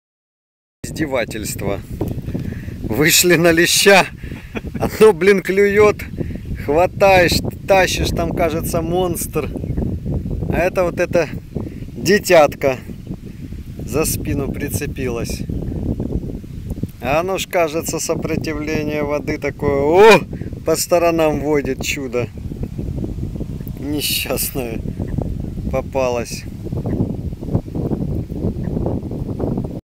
Случай на рыбалке 🦈
Случай на рыбалке - ловим леща на фидер и пенопласт, тащу, сопротивляется, а тут...

▶ Не пожалейте пару минуток, для вас стараюсь, мои дорогие зрители! Приглашаю посмотреть мои каналы, наверняка найдёт каждый для себя интересные видео: